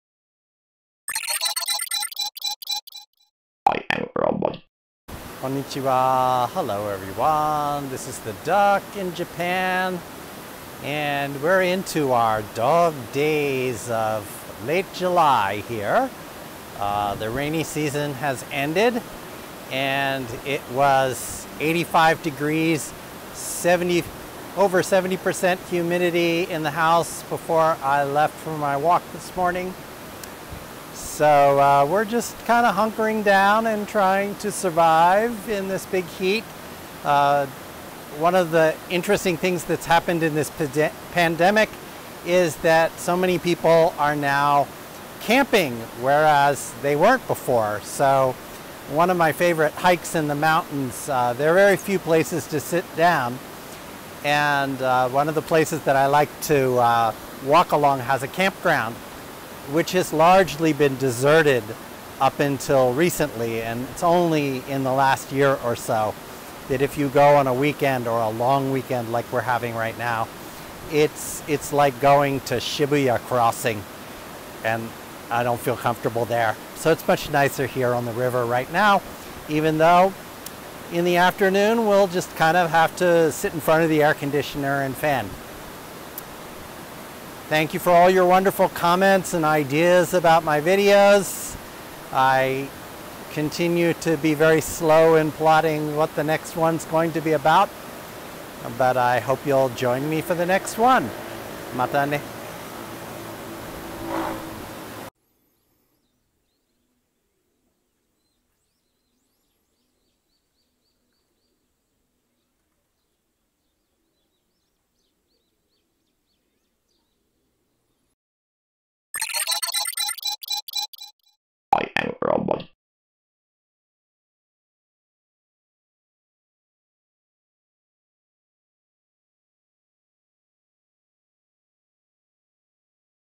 River rapids (210703)
We're into our dog days of late July here.

The rainy season has ended and it was 85 degrees and over 70% humidity in the house before I left for my walk this morning. So we're just kind of hunkering down and trying to survive in this big heat.

Due to the pandemic, many people are now camping. There's a campground on one of my favorite hikes in the mountains that's like Shibuya Crossing now. I don't feel comfortable there.

It's much nicer here on the river right now even though in the afternoon we'll just kind of have to sit in front of the air conditioner and fan.

Thank you for all your wonderful comments and ideas about my videos. I continue to be very slow in plotting what the next one's going to be about, but I hope you'll join me for the next video.